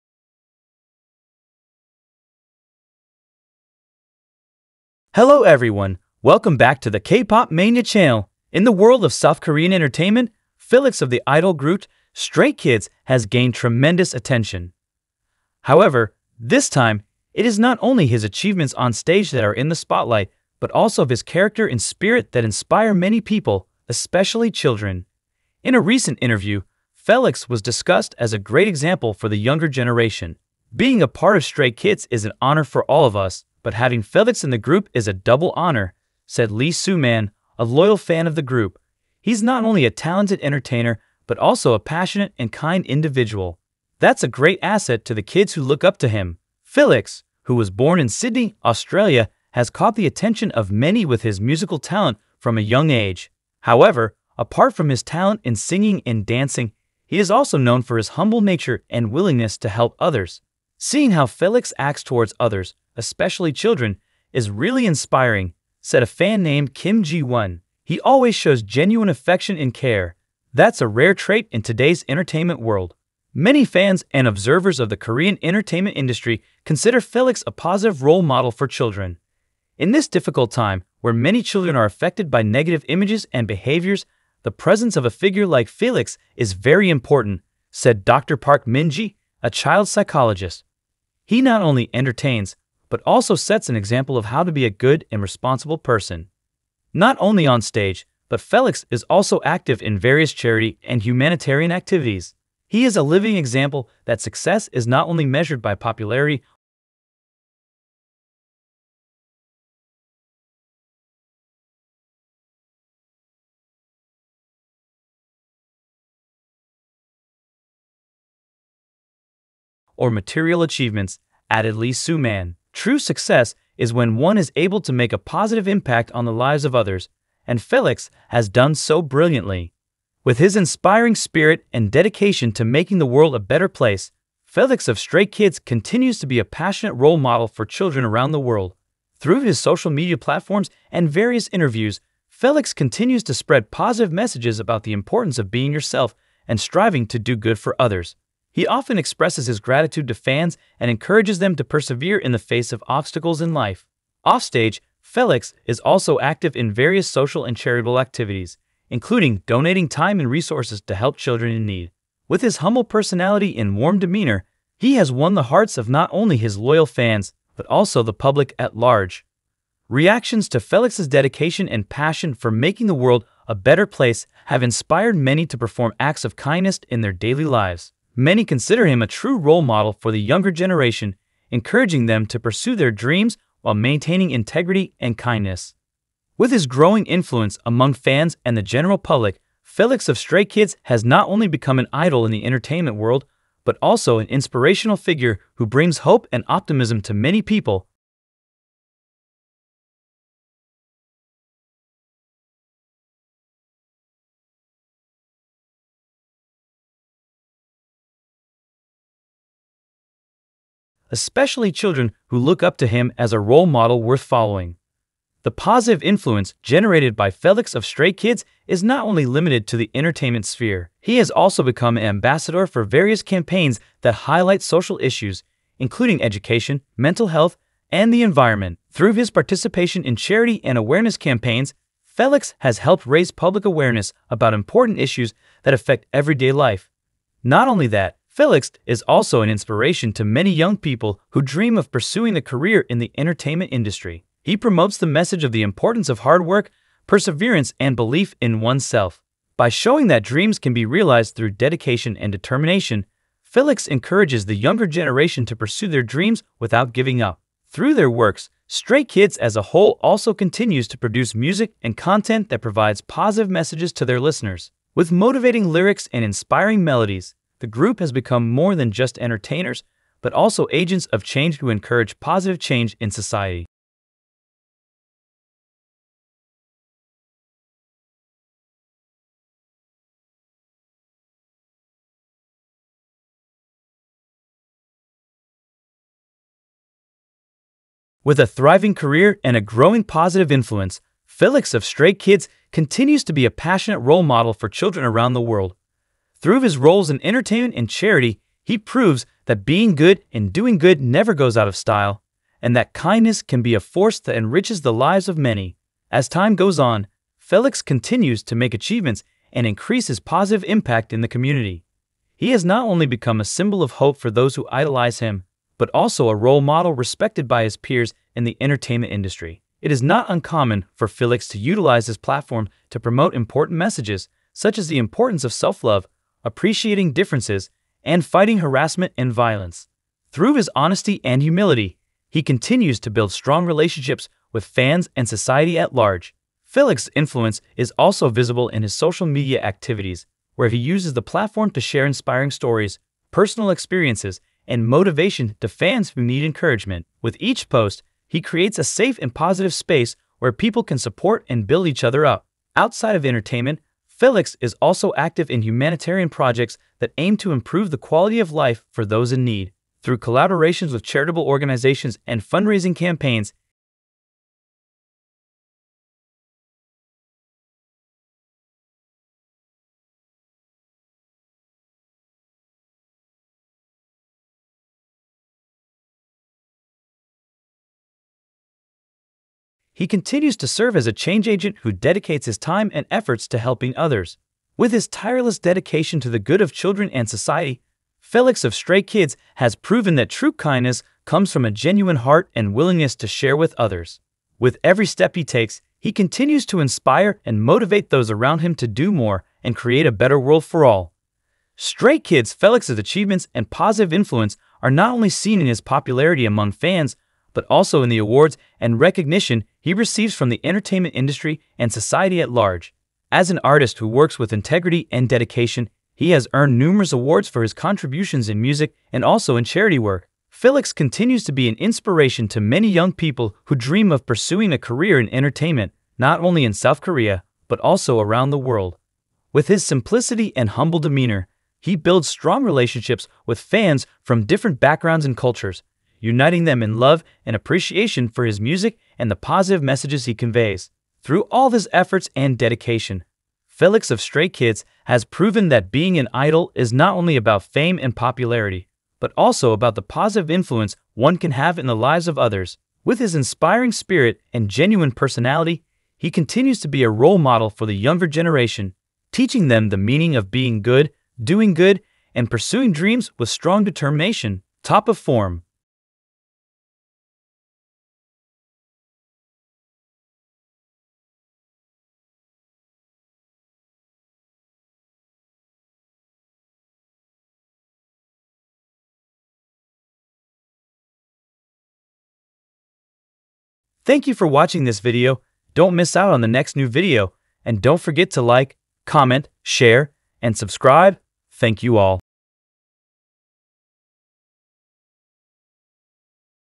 Unexpected!!! Felix Straykids Symbolizes Young Generation's Hope in Realizing Their Dreams
With a thriving career and a growing positive influence, Felix of Stray Kids continues to be a passionate role model for children around the world. Through his roles in entertainment and charity, he proves that being good and doing good never goes out of style, and that kindness can be a force that enriches the lives of many.